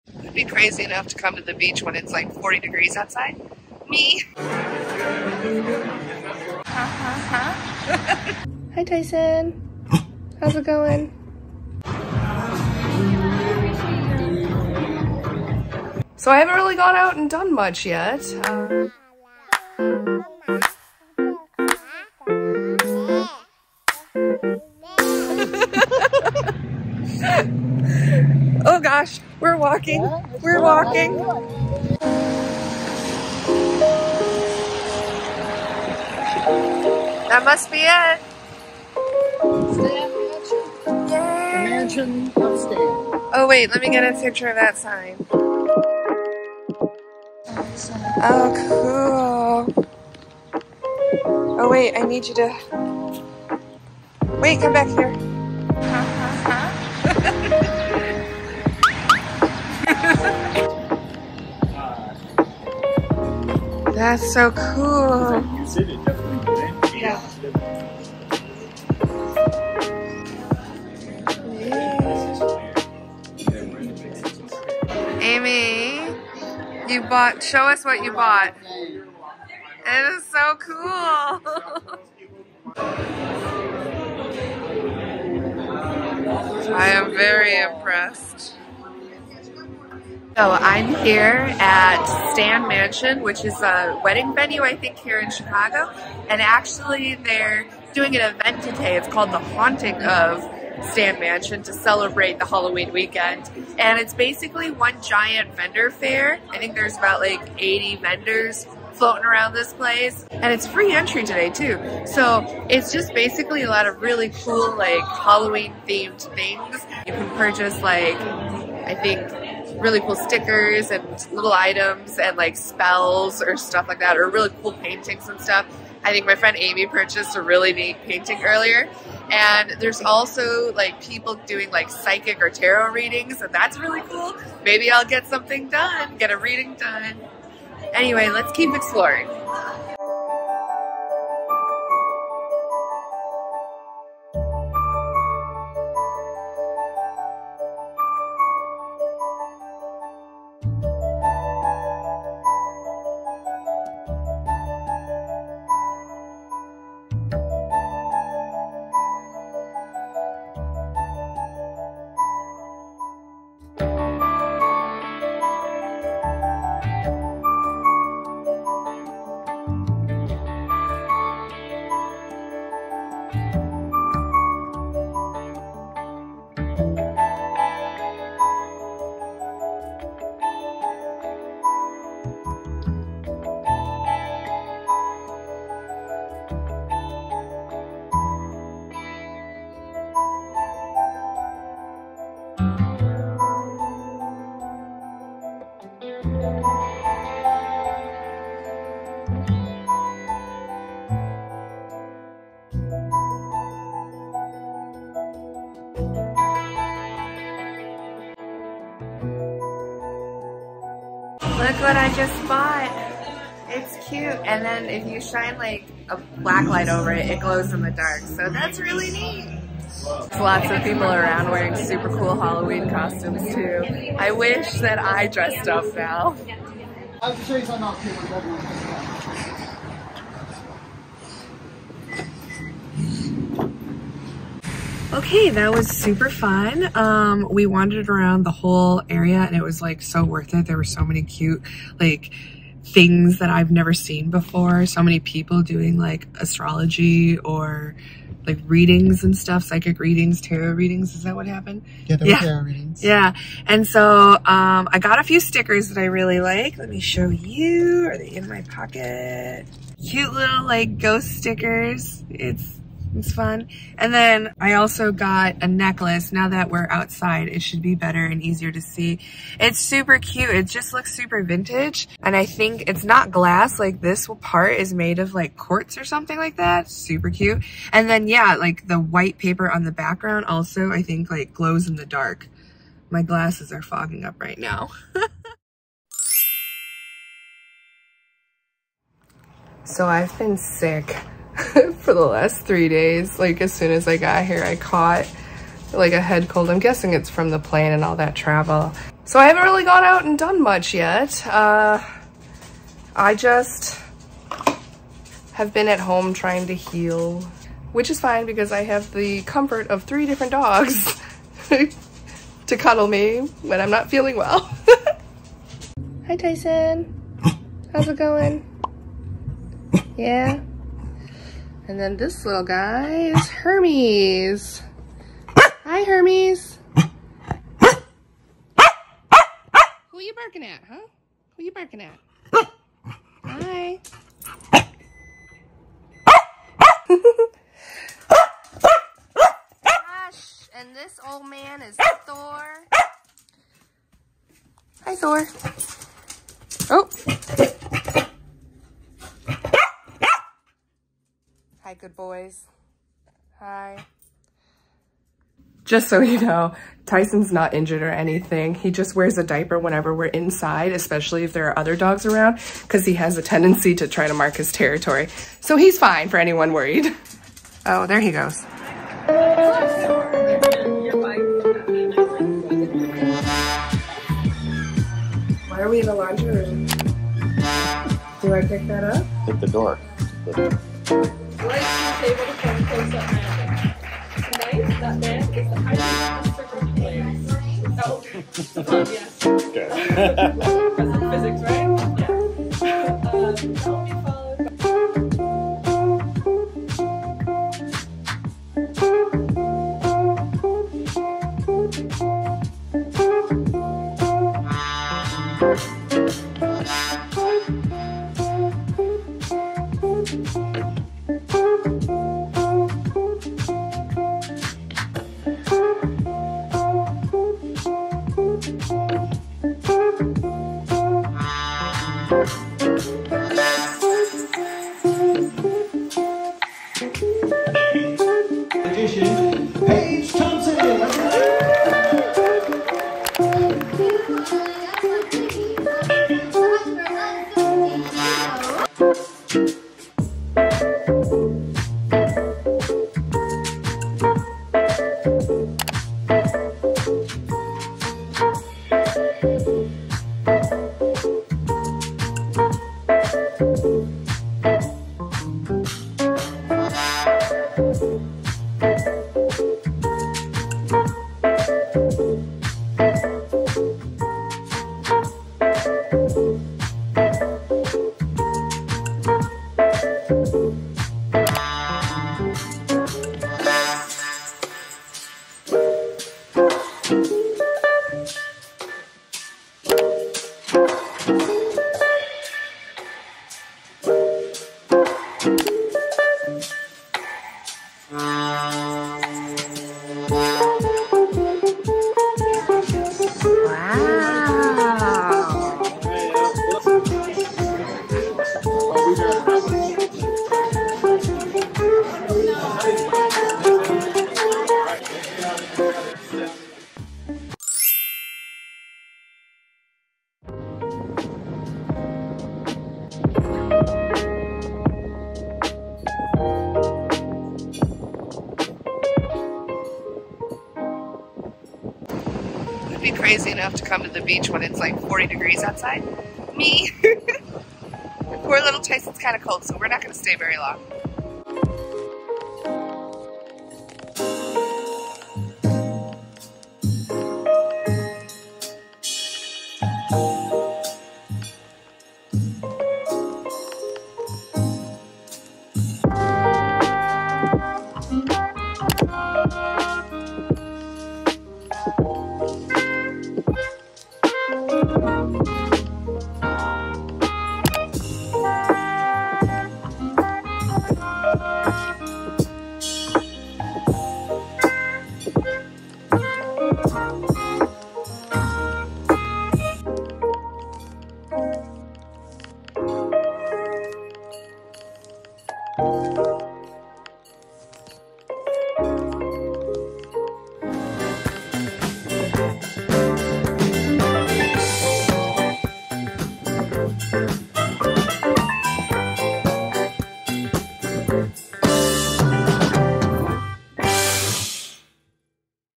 Chicago diaries: self care and romanticizing my life
Spend a casual week in Chicago with me and Tyson :)

Jackery Portable Power Station Explorer 1000
ROCKPALS 100w Foldable Solar Panel
Car Magnetic Window Sun Shades